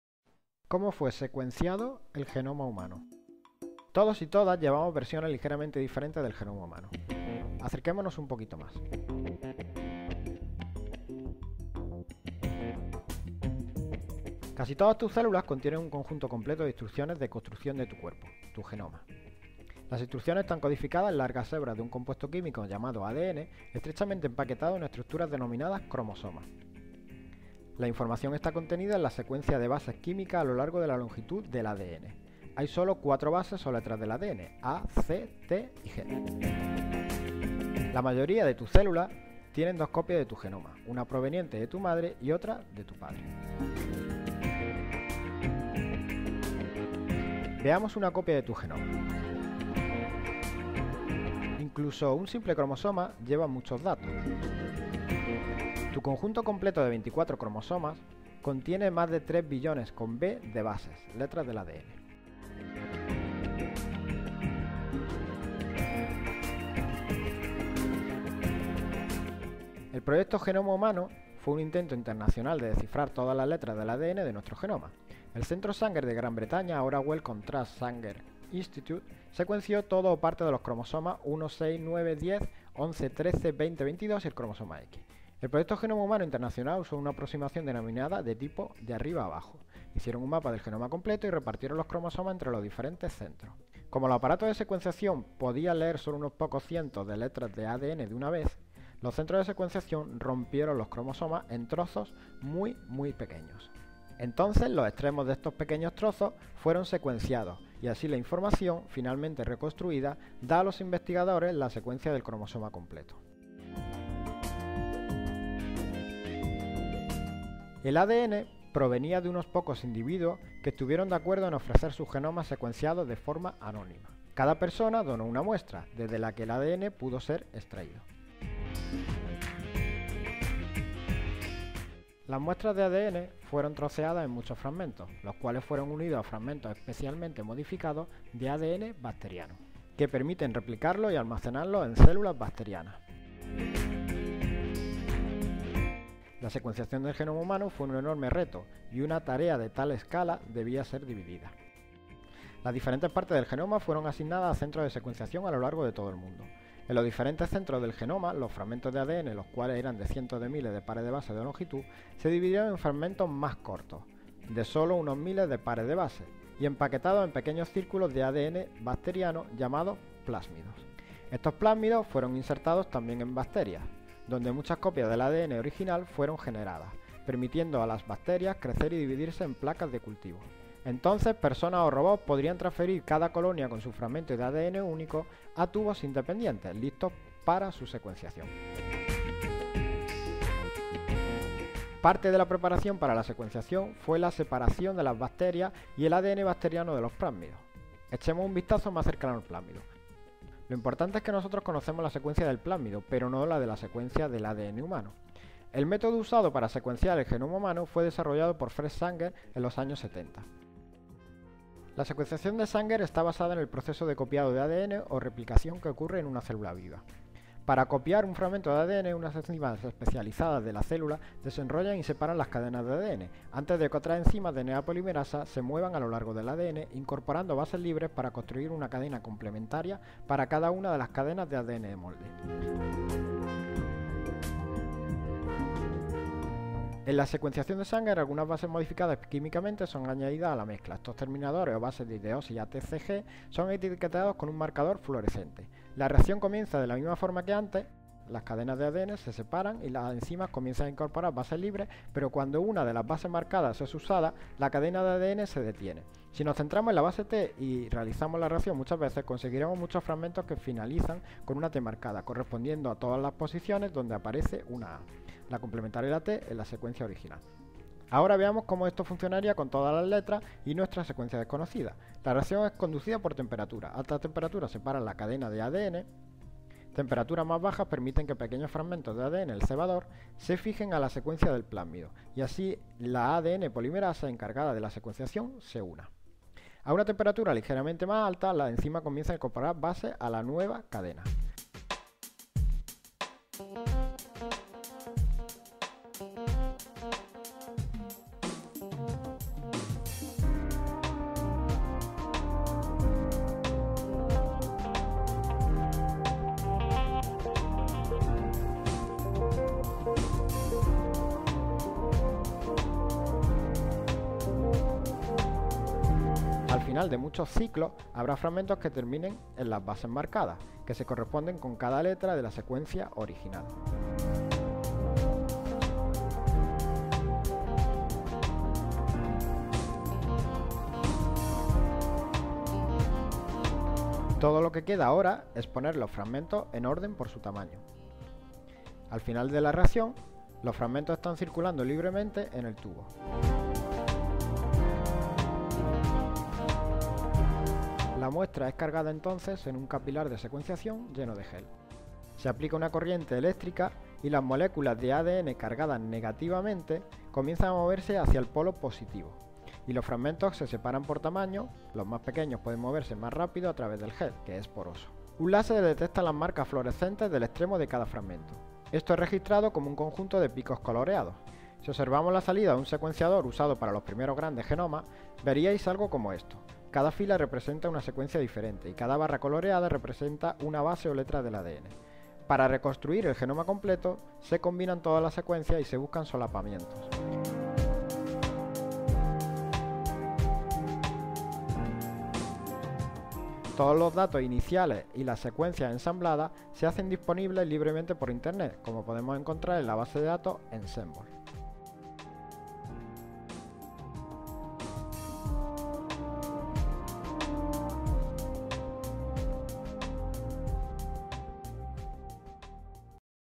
Como fue secuenciado el genoma humano
Vídeo doblado al español sobre como se secuenció el genoma humano.